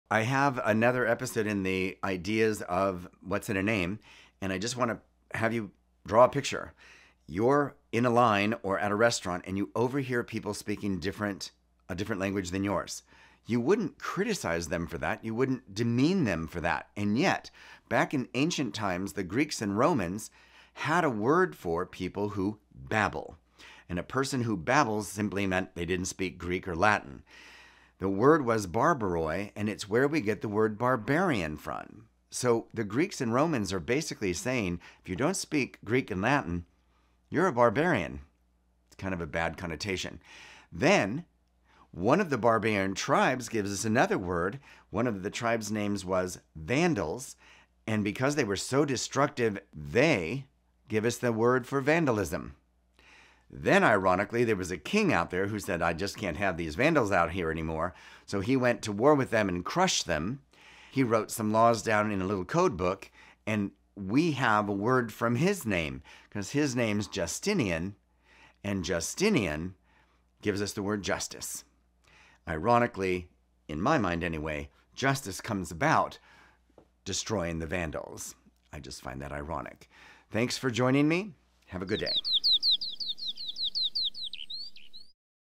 What's in a Name - Barbarians, Vandalism, and Justice
In today's episode we discuss the origin of the words Barbarian, Vandalism, and Justice.